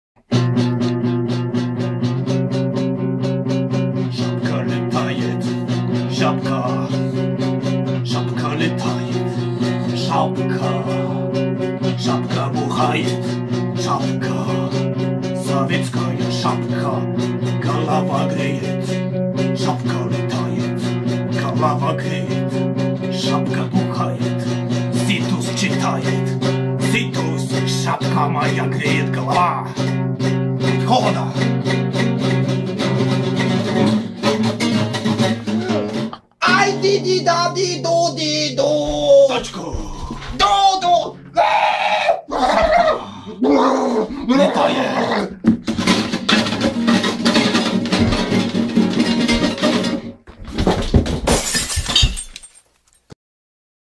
История одинокой шапки
Видео снималось в день лиго. 23 июня. — латышский народный праздник. Отмечается обычно в ночь с 23 на 24 июня и считается одним из самых больших и популярных праздников в латвии. День перед Лиго называют Зелёным днём. Все выезжают на природу, разводят костры, поют песни характерные только для этого праздника.